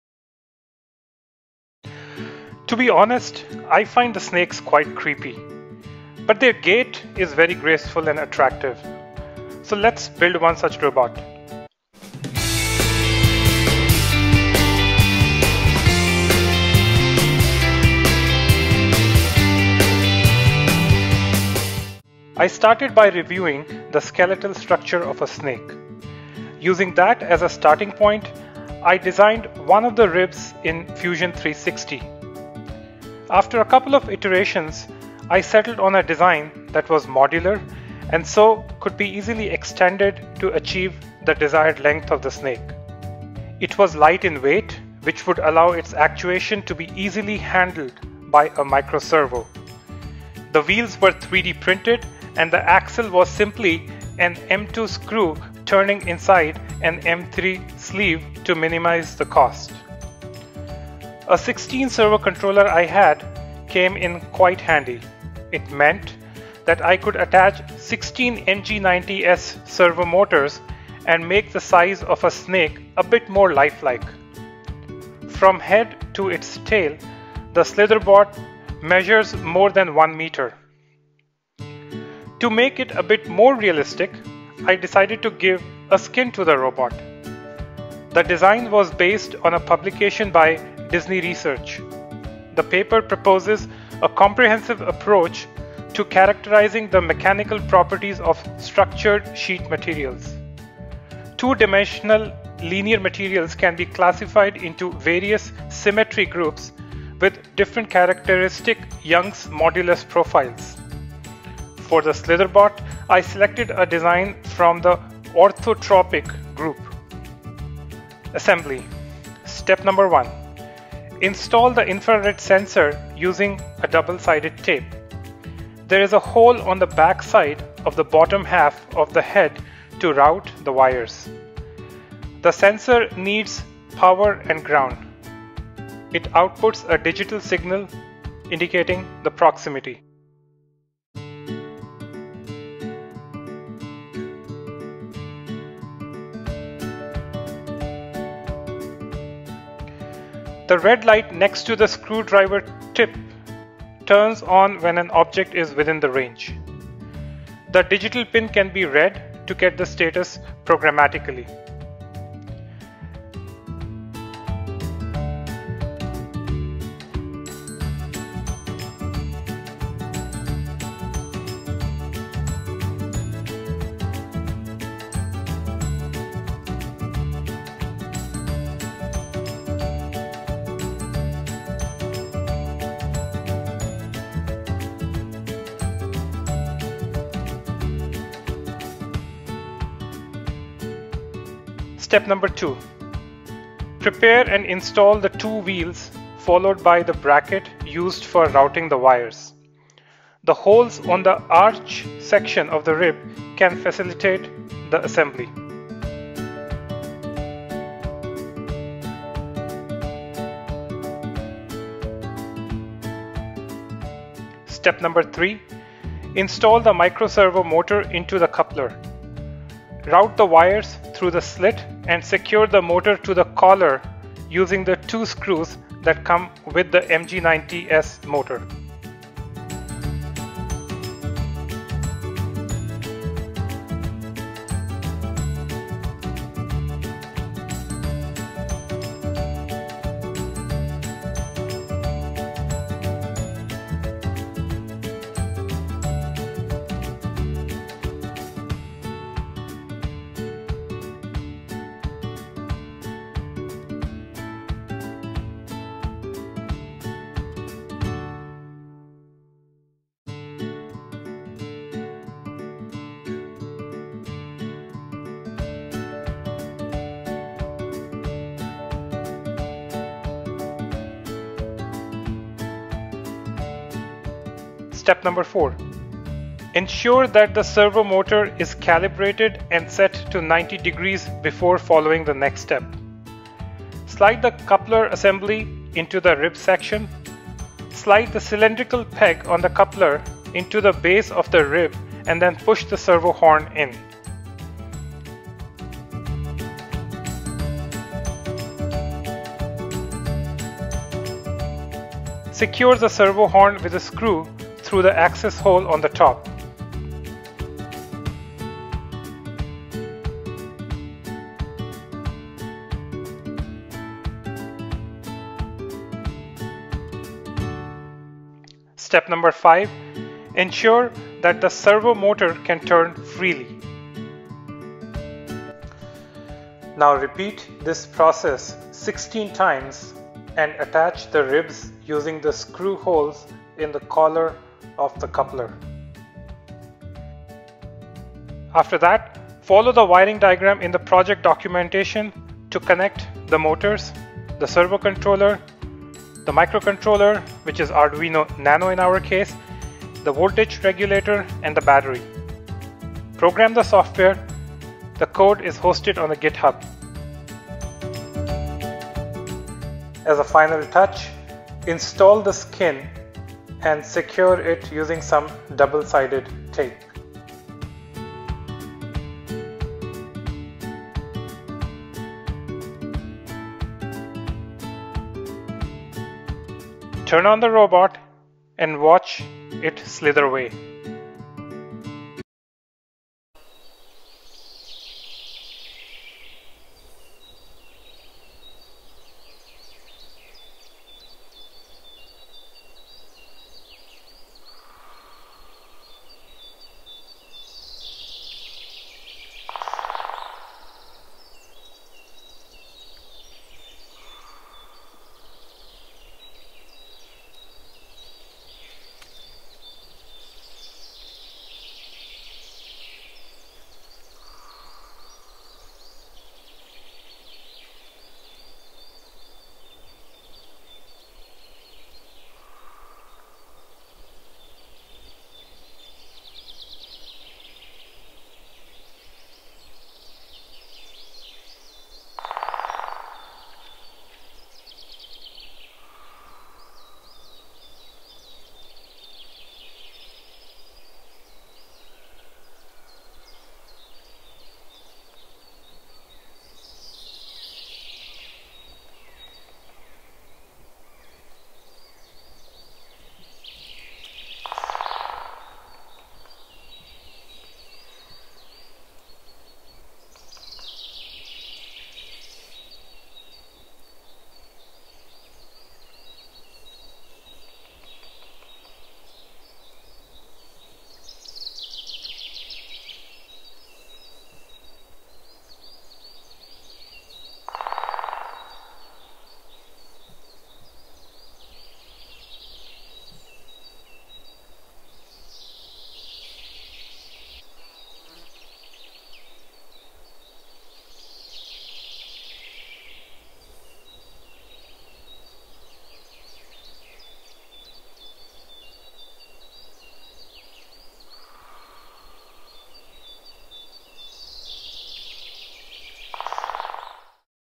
Slitherbot
A Snake robot with 3D printed skin actuated by micro-servos.